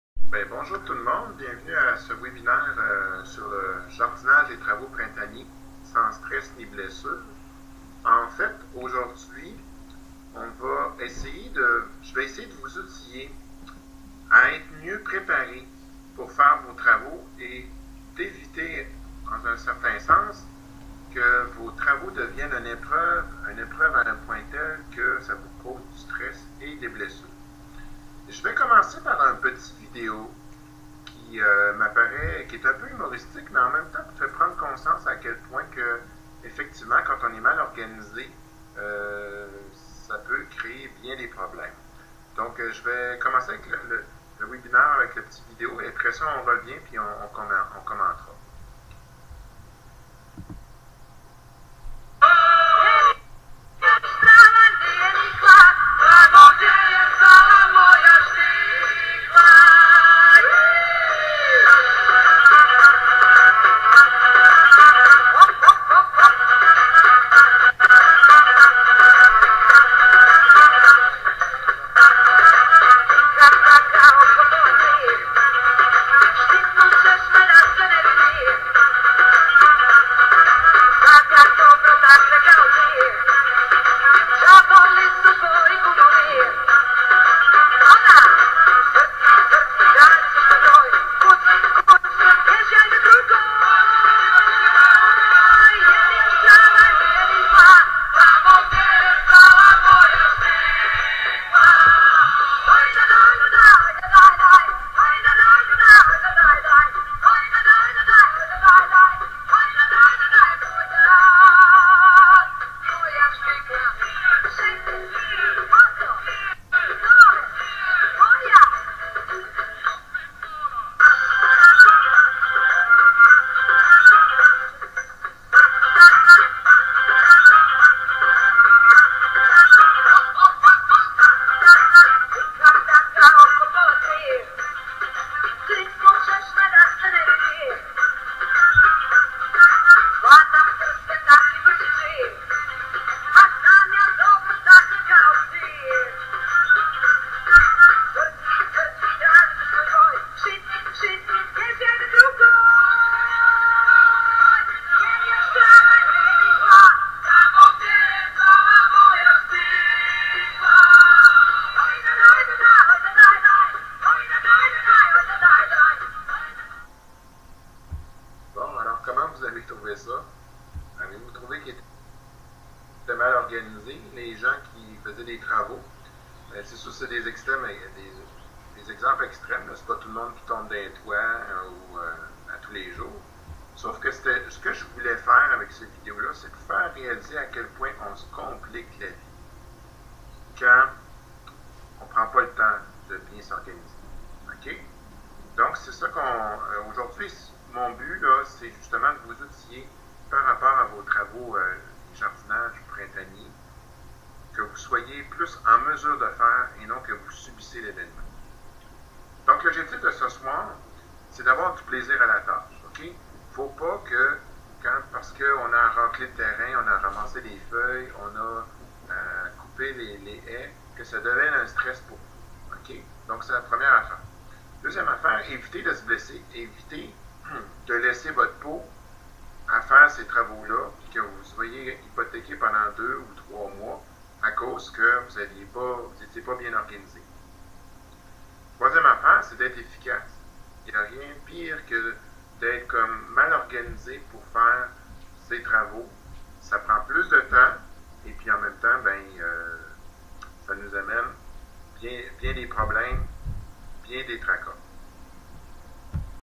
Jardinage
Comment ne pas se blesser en jardinant? Dr Lachance, chiropraticien, répond à vos questions.